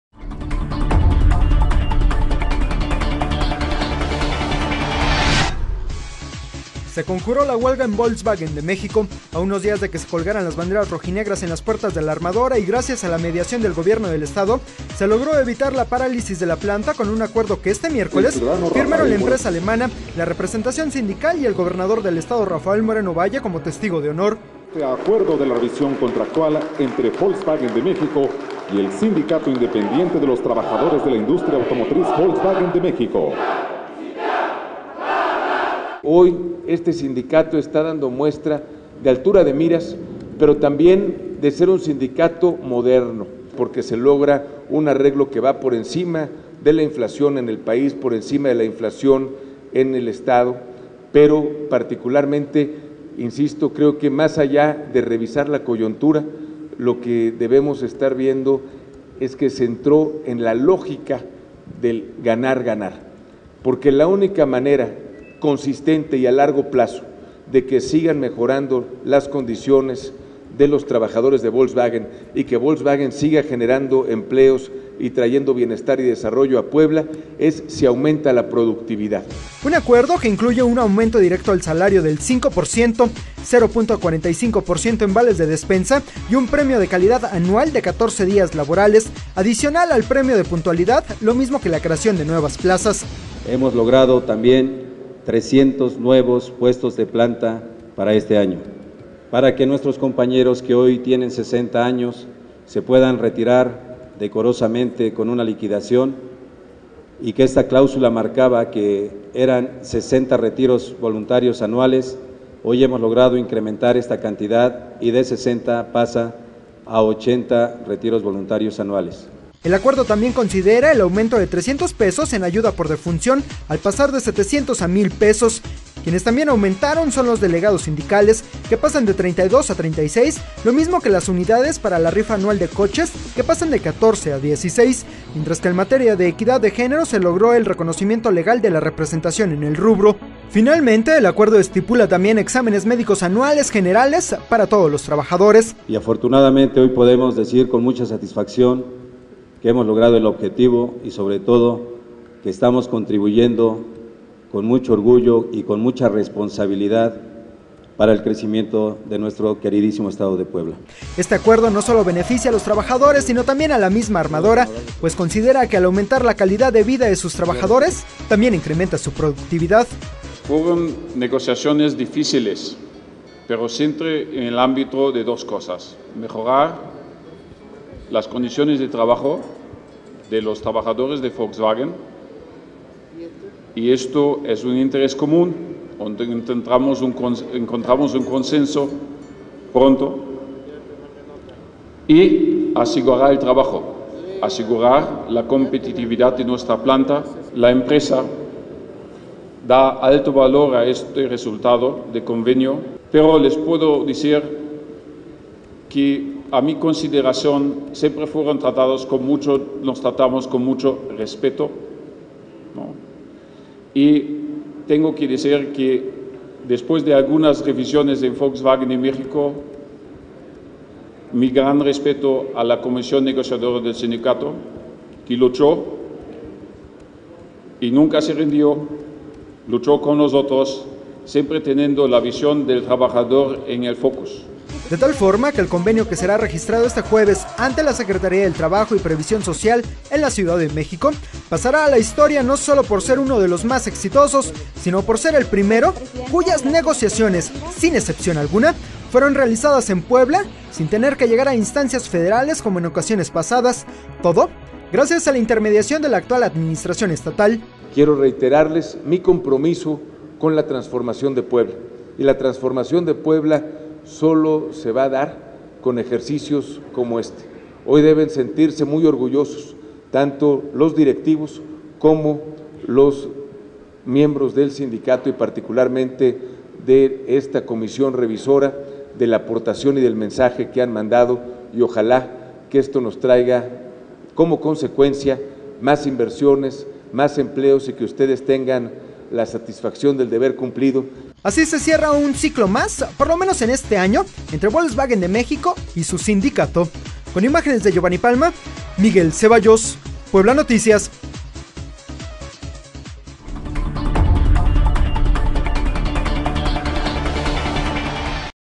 Aumento salarial de 5% y en prestaciones .45% a trabajadores de VW
Atestigua la firma del contrato de revisión salarial el gobernador RMV